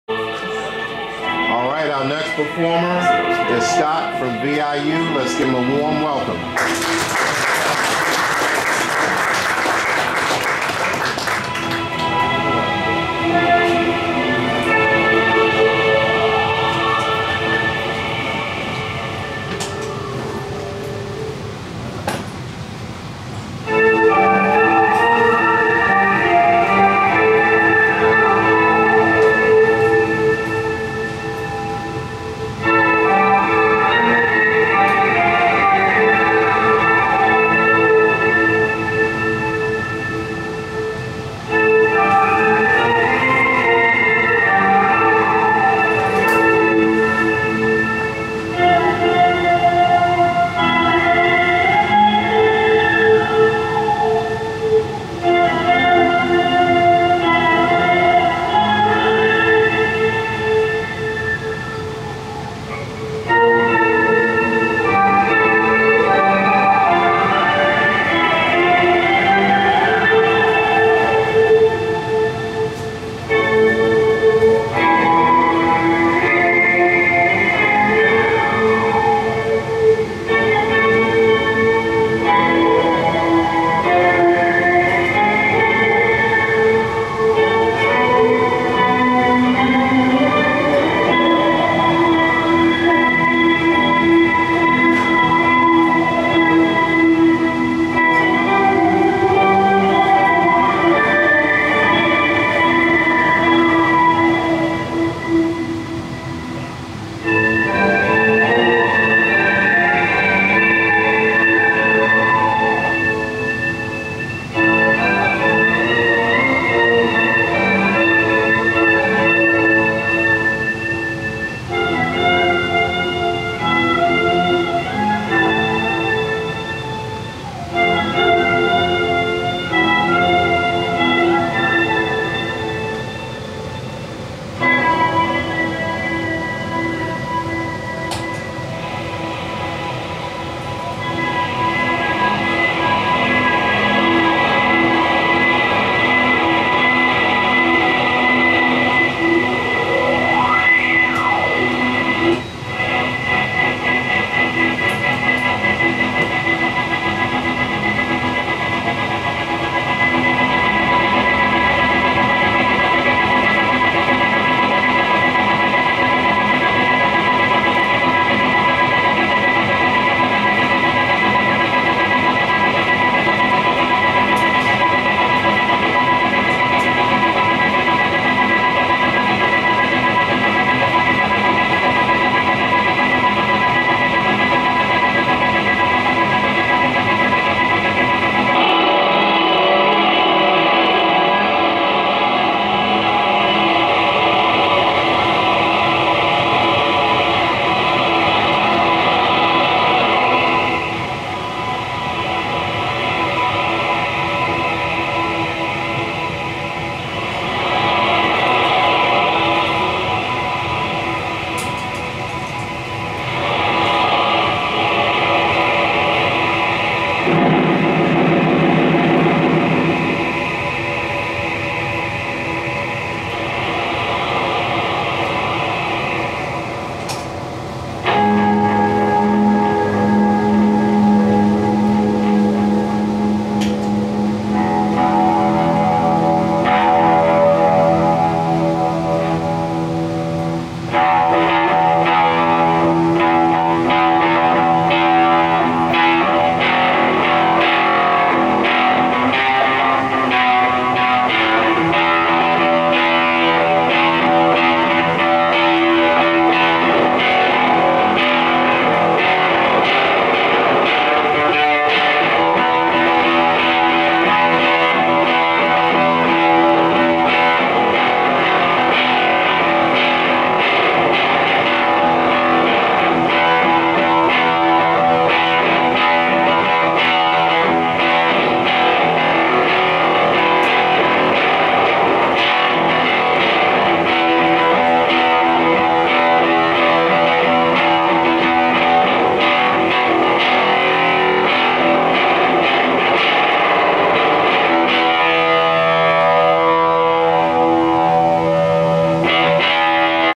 Scott Mitzner's performance on 02/23/2019 (Hi-Def video 2 of 5)
This is a short promotional video featuring Scott Mitzner "VIU"

Baltimore Electronic Music 2019 Winterfest Concert One at the Electric Maid in Washington DC on 02/23/2019

Logan Mitchell Sr - Baltimore SDIY Group's leader & founder
Videos Produced by Synth Tech Project Productions